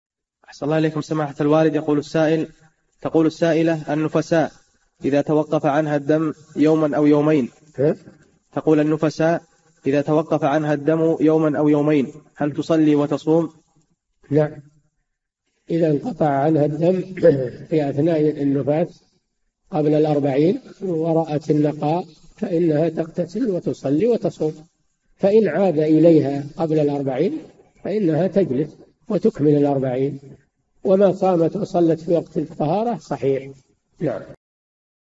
[515- 4717] هل تجب الصلاة على النفساء إذا طهرت قبل الأربعين - الشيخ صالح الفوزان
"[4717-515] أحسن الله إليكم سماحة الوالد تقول السائلة: النفساء إذا توقف عنها الدم يومًا أو يومين هل تصلي وتصوم؟
الجَوَابُ: نعم إذا انقطع عنها الدم في أثناء النفاس قبل الأربعين ورأت النقاء فإنها تغتسل وتصلي وتصوم، فإن عاد إليها قبل الأربعين فإنها تجلس وتكمل الأربعين وما صامت وصلت في وقت الطهارة صحيح.

للاشتراك في قناة الشيخ صالح الفوزان
فتاوى المجالس في تفسير المفصل

للاستفسار والإقتراحات والدعم
رئيس الفريق
لمشروع كبار العلماء